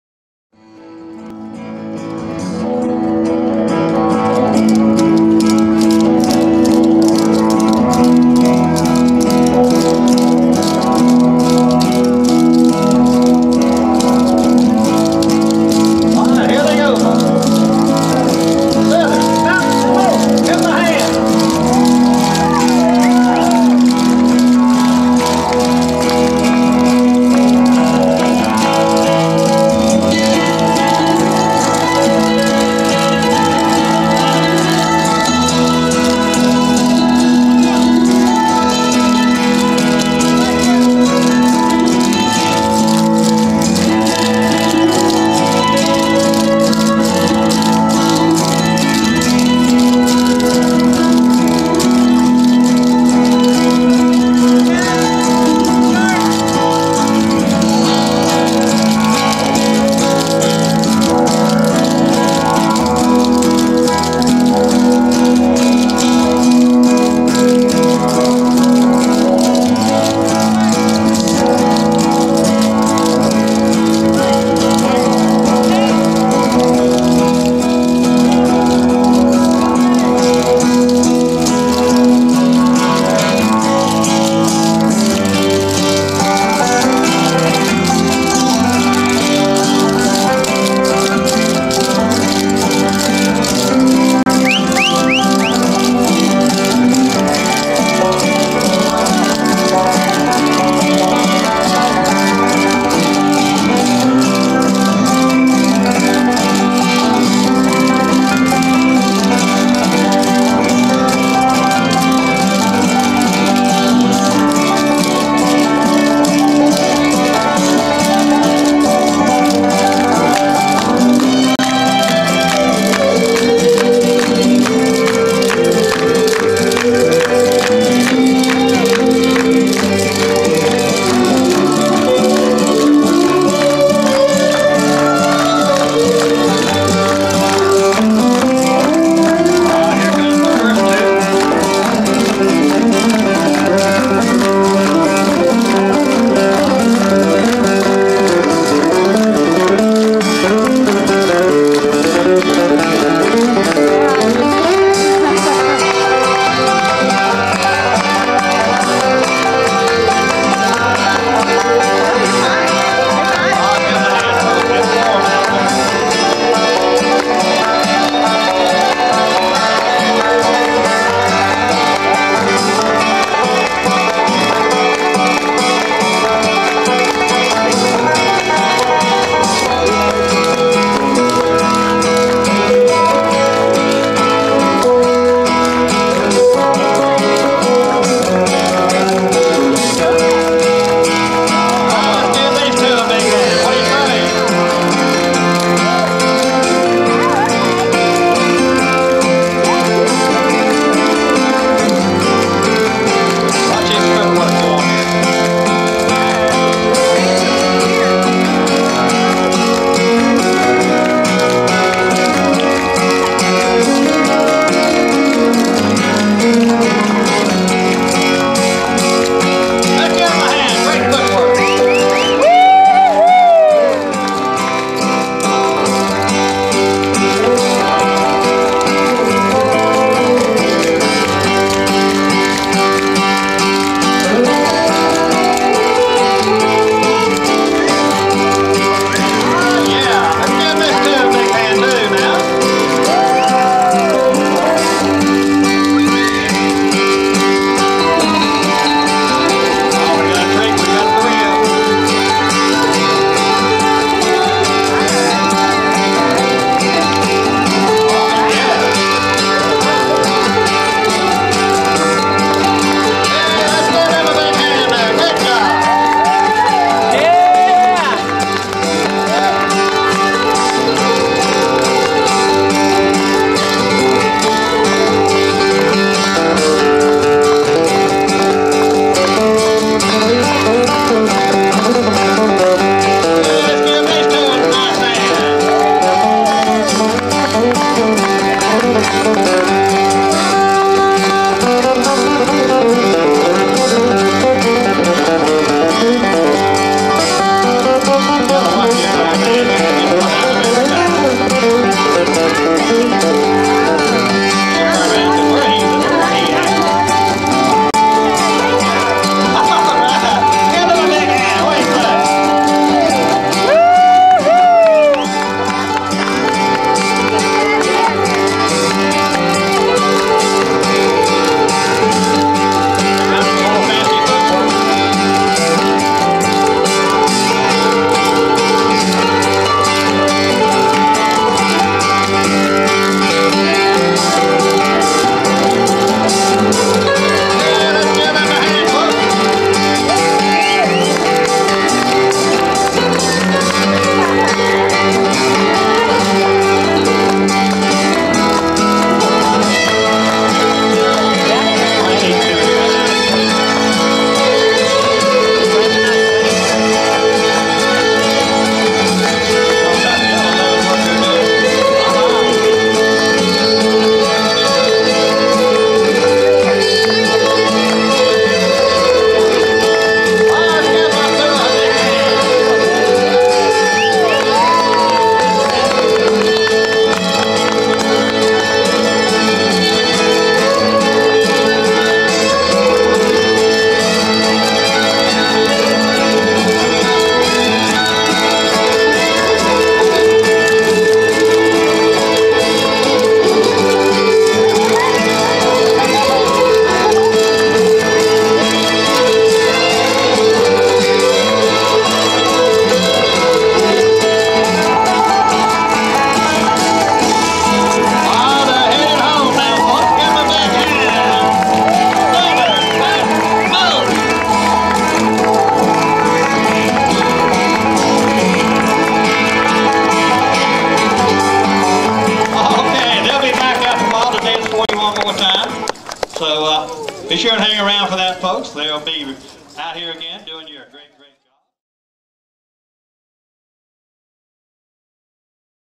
h'ville cloggin 8:11.mov
Hendersonville Street Dance on Monday nights included Steve Tallent's Southern Mountain Cloggers on 7/11.